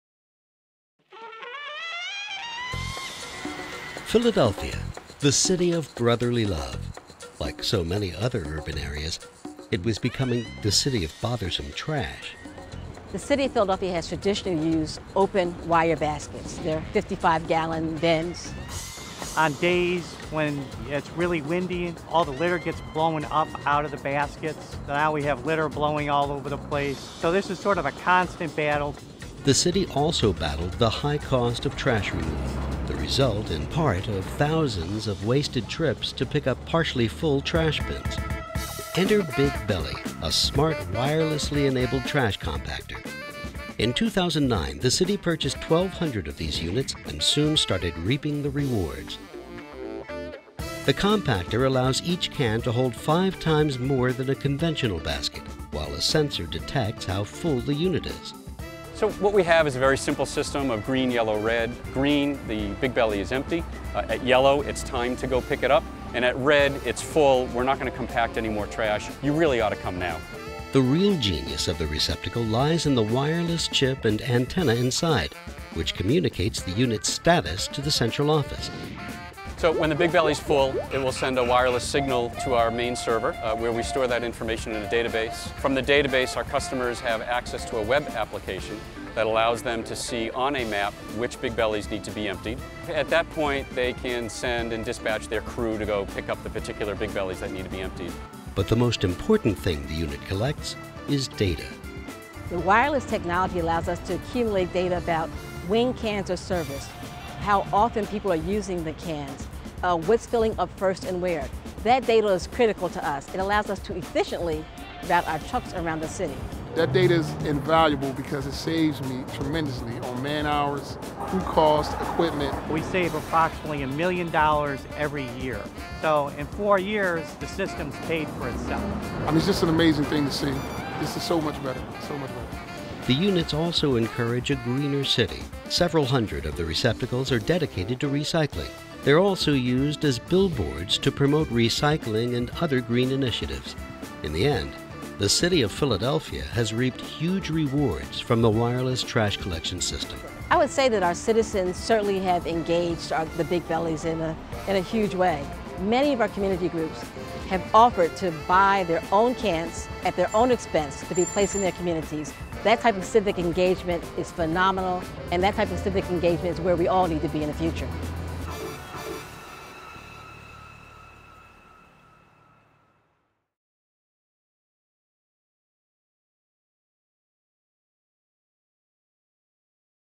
Wireless is Limitless: Self-Reporting Trash Bins
Municipalities are giving their waste removal systems a major efficiency upgrade. They're using trash and recycling bins that send a wireless signal when they are ready for pickup, and trucks are only dispatched when necessary, saving cities millions in operating costs.